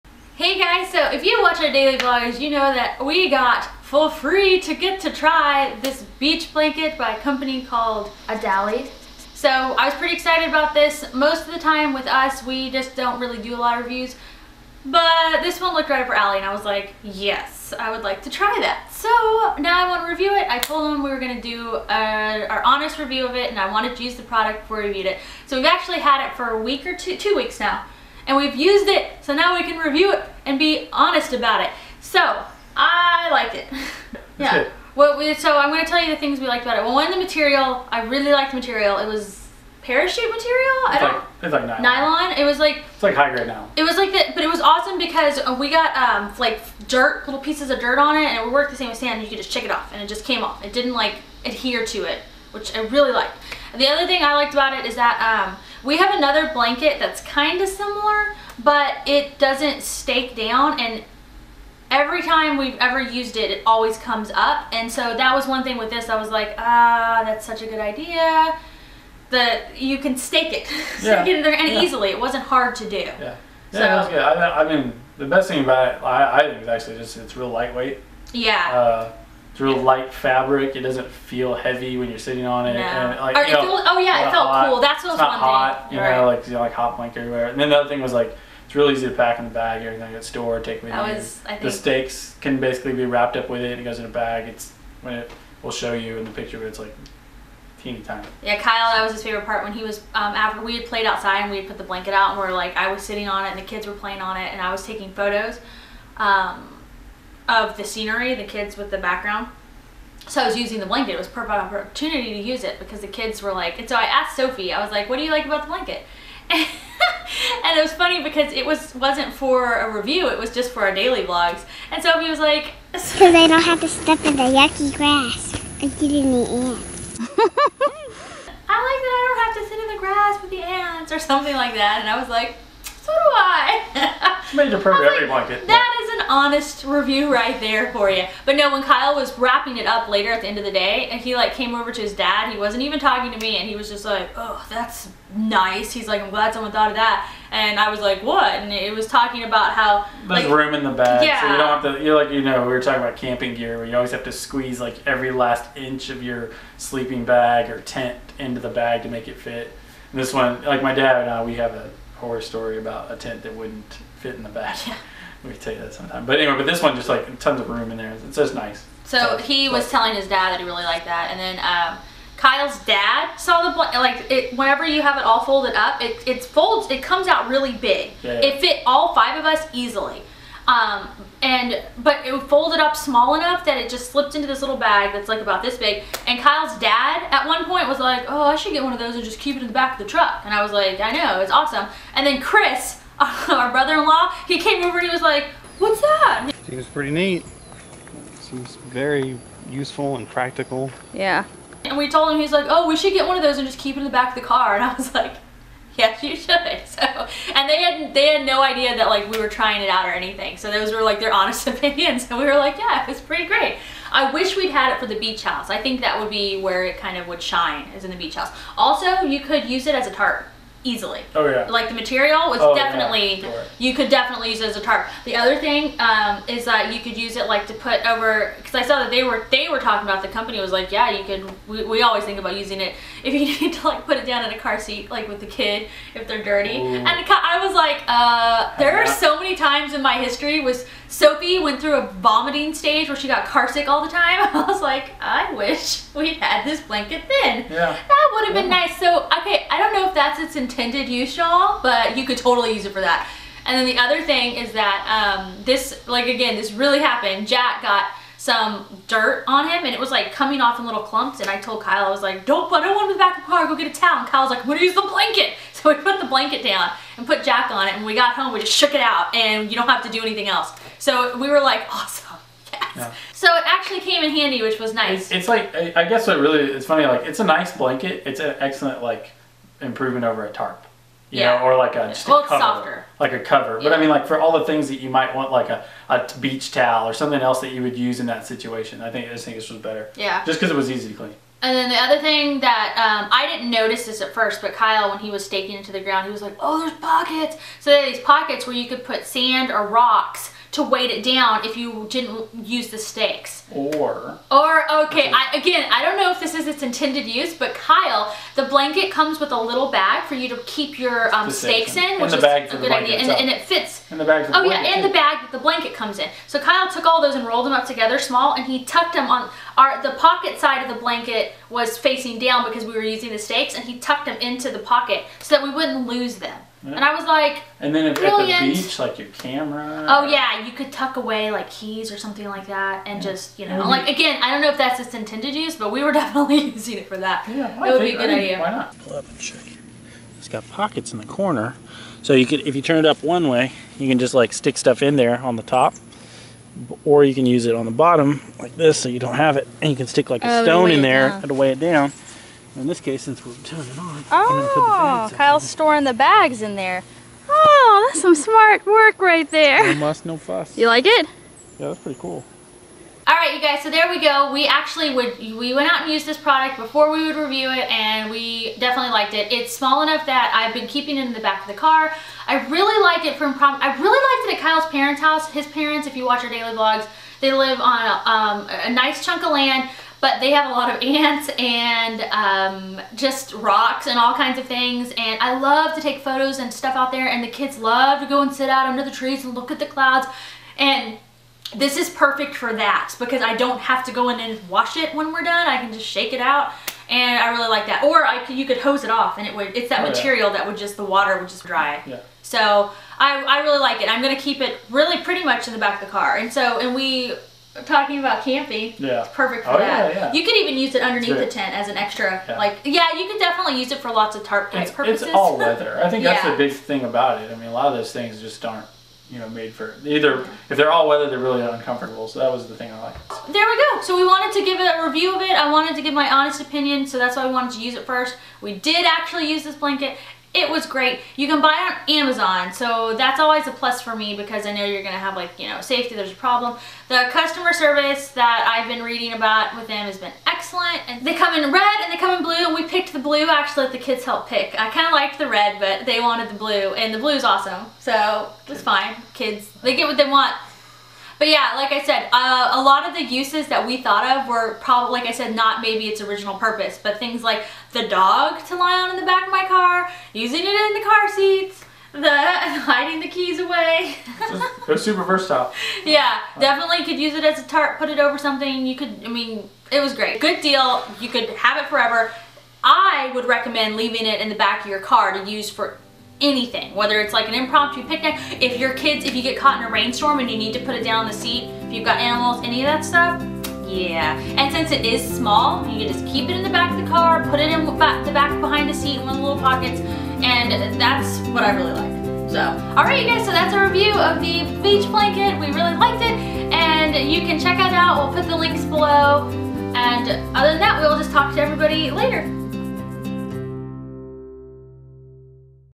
Adalid Gear Beach Blanket Review
We received this blanket for free but tried it out for two weeks first so we could give you our honest opinion. We actually found it to be quite helpful and not always for the obvious reasons. =p My favorite thing about it was how easy it was to clean and that is was the perfect size to keep in the car. Kyle's favorite thing was how easy it was to put away and the little hidden pockets. Sophie liked that she could lay outside and not have to be bothered by ants, hehehe!

(PS: It is on sale right now!)